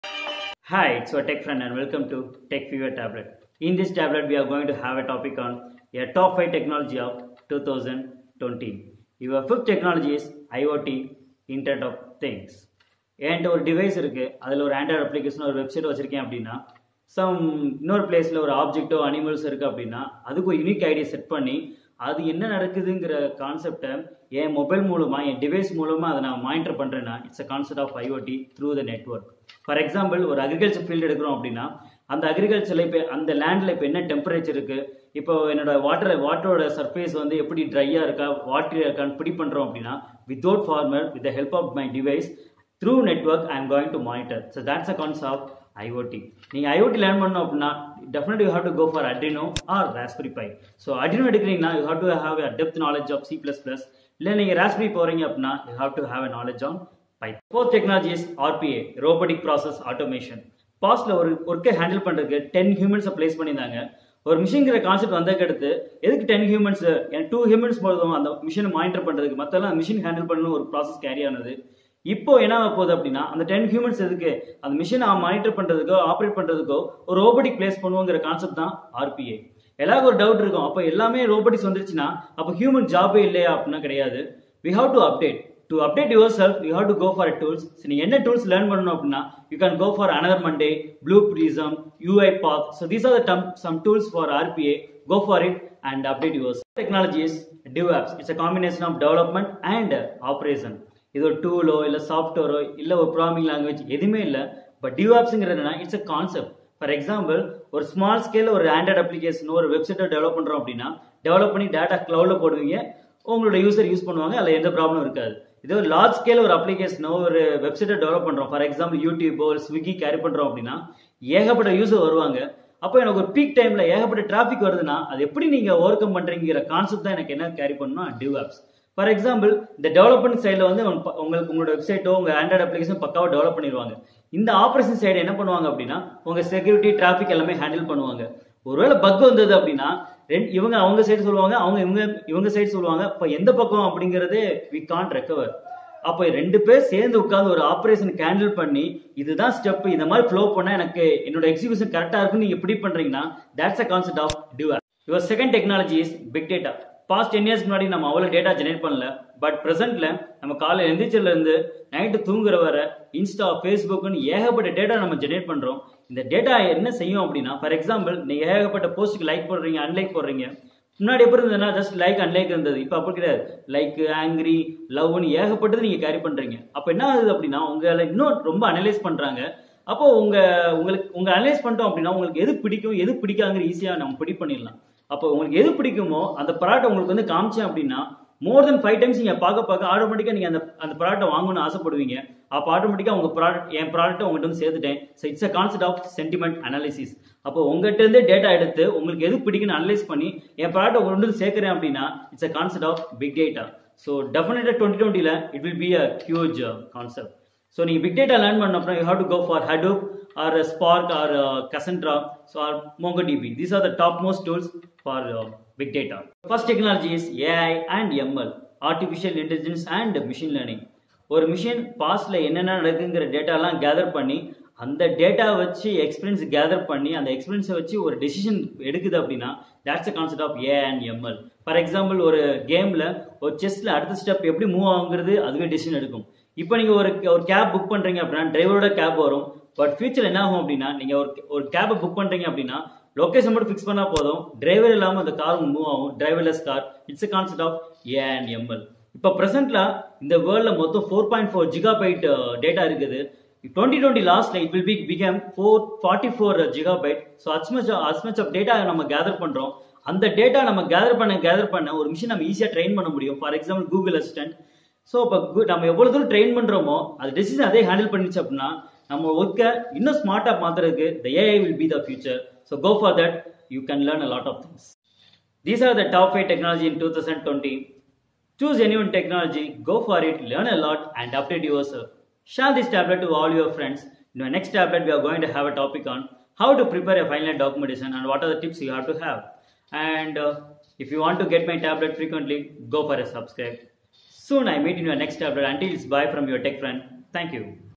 Top 5 Technology of 2020 in Tamil
This video talks about the top 5 technologies to learn in 2020.

To place a unique candidate in IT Sector, start a year with any one of the technology.

This tablet is going to provide a medicine for your tech fever. This video helps you select the perfect recent platform. All category of students with computer background such as EC, EEE, Mech, CS, IT, MCA, MSc, BCA, BSc can make use of this video in an effective manner.

Get More Ideas on Latest Technology, Click the below link